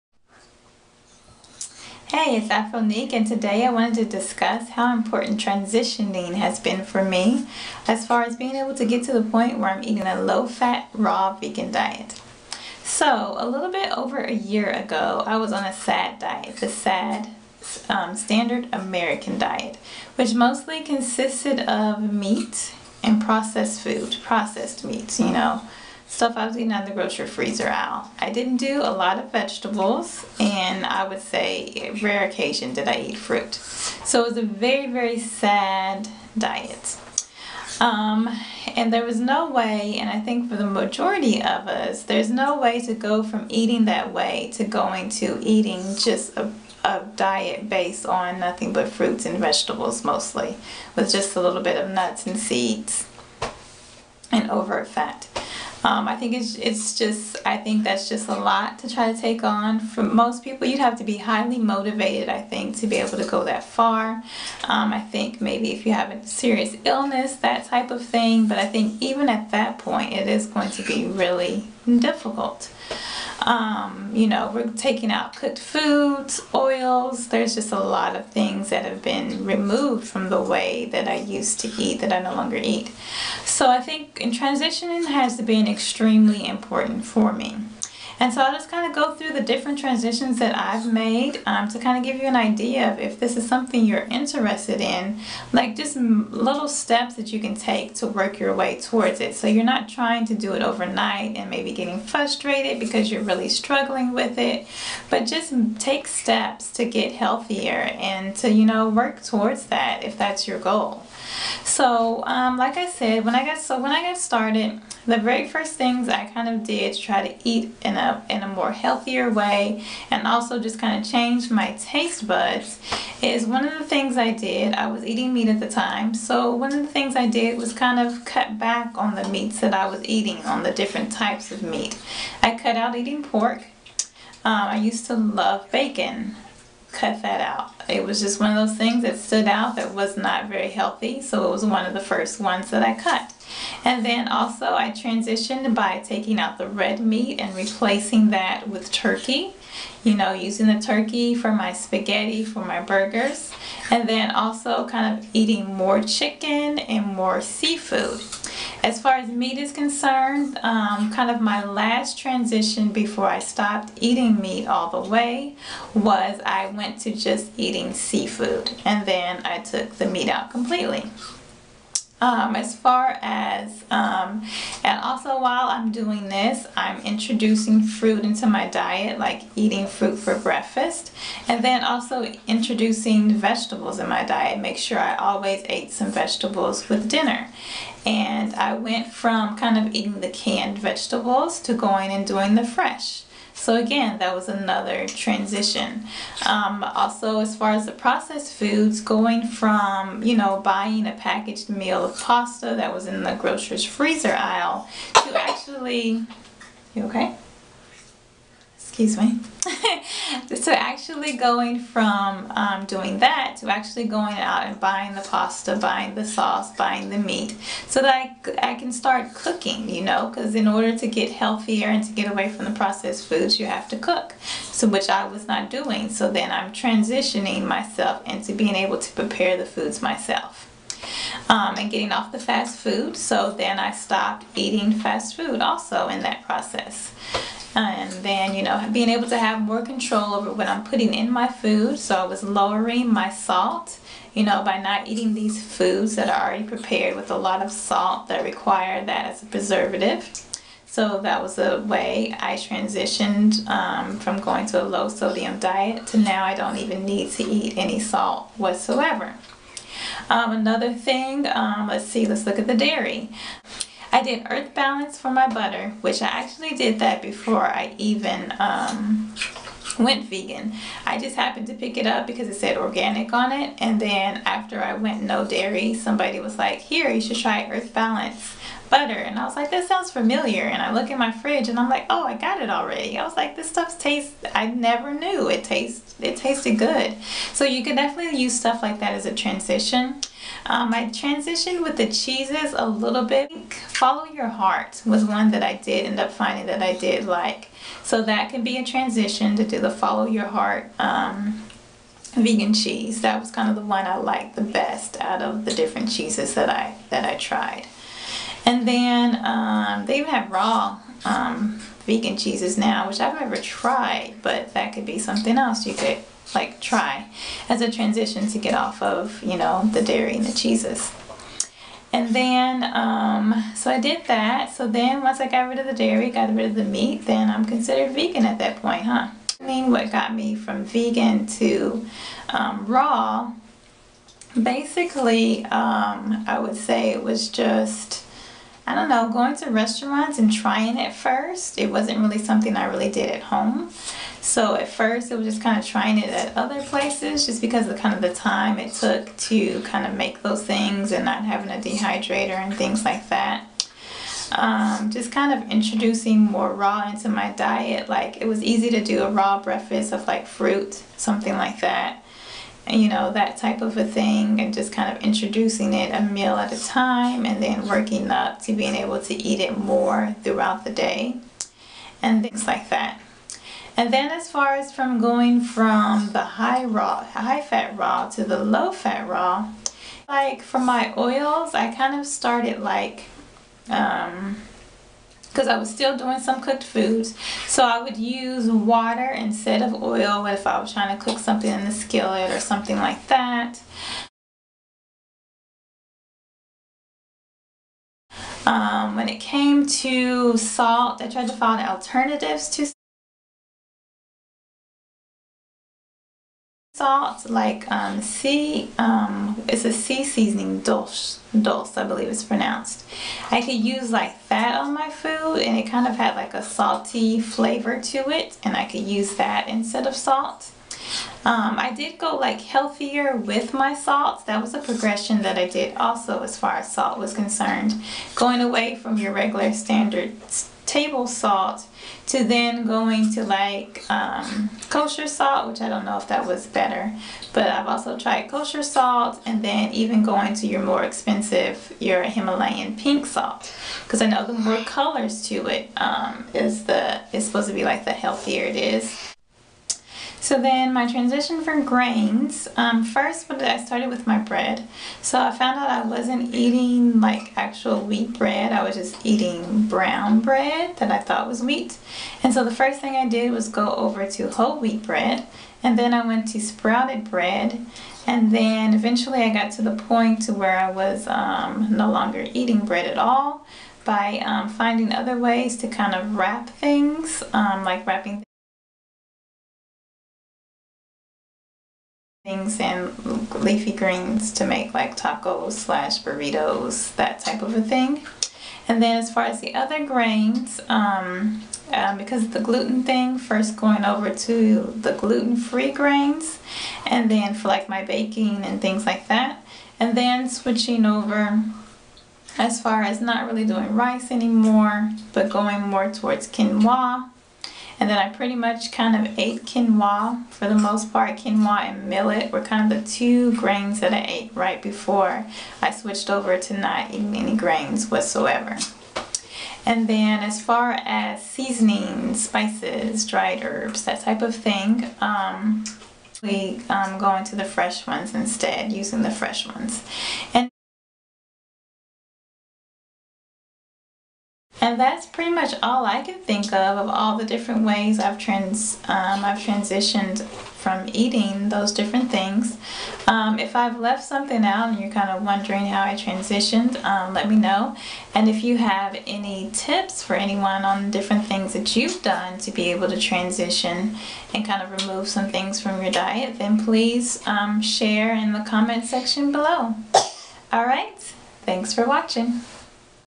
Making Transitions To Eat Healthier
Steps I took to eat healthier that took me from eating the standard American Diet (S.A.D) of meat, processed foods, dairy, and salt to a low fat raw vegan diet of fruits, vegetables, and minimal overt fats, nuts and seeds.